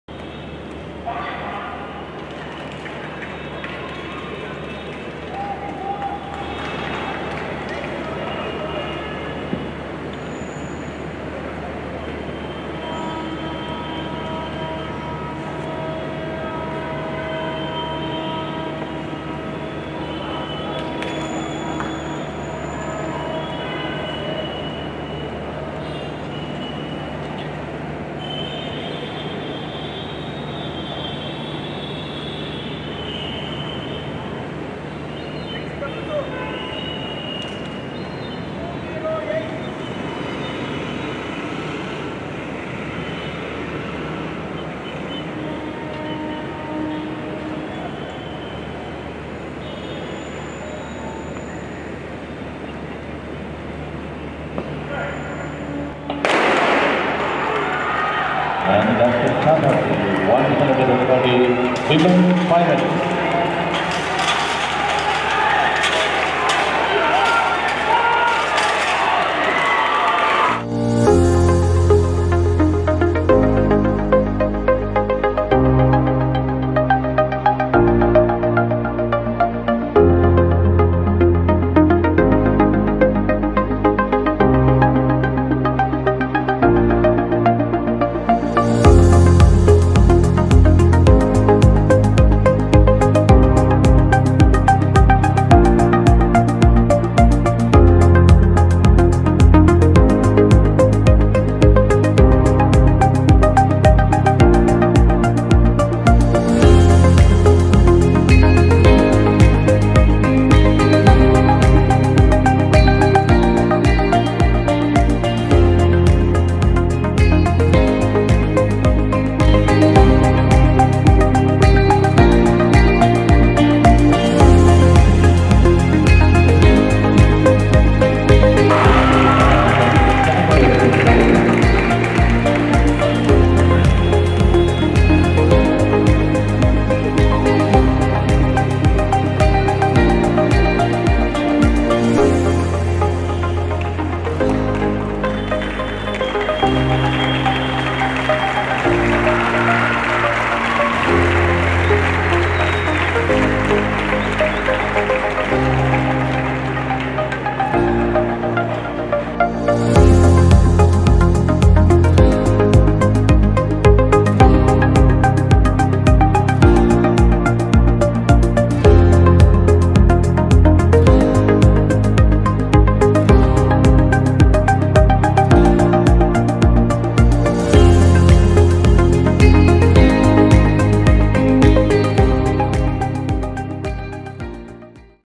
100m Hurdles Final | The Most Motivational Video of All Time | 61st Interstate Senior Athletics 2022
Event # 29 WOMENS 400m Hurdles
& C.W.G Q.MARK: 55.55

# NR:P.T USHA LOS ANGELES 55.42 08/08/1984
% MR:JAUNA MURMU GUWAHATI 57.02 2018

RESULTS OF FINALS

1 R VITHYA RAMRAJ 516 ATNF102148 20/09/98 TAMIL NADU 57.08

2 ANU R 318 AKLF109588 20/04/93 KERALA 58.99

3 ARATHI R 317 AKLF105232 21/05/01 KERALA 59.26

4 DHIVYA.J . 522 ATNF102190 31/01/98 TAMIL NADU 1:00.08

5 SIMMY . 193 AHRF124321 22/09/03 HARYANA 1:00.73

6 GUDIYA . 603 AUPF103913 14/09/98 UTTAR PRADESH 1:00.75

7 BHATERI . 191 AHRF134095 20/05/96 HARYANA 1:01.31

8 SAYANA P.O . 319 AKLF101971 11/03/98 KERALA 1:03.25